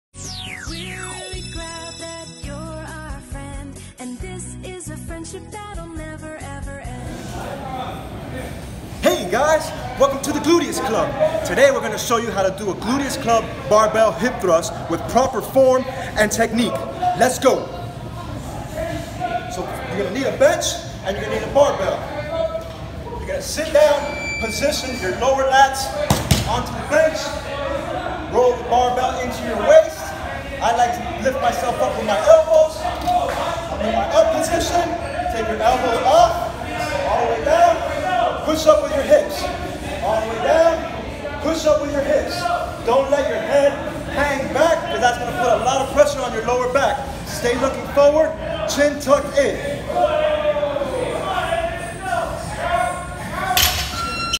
How - to: Barbell Hip Thrust
a tutorial on how to perform a gluteus club barbell hip thrust with proper form and technique.

Tips: lower lats on bench, barbell at waist, feet bent with knees positioned behind toes, chin tucked in looking forward, feet shoulder width, don't let knees cave in, push through heels and hips, and squeeze butt at top.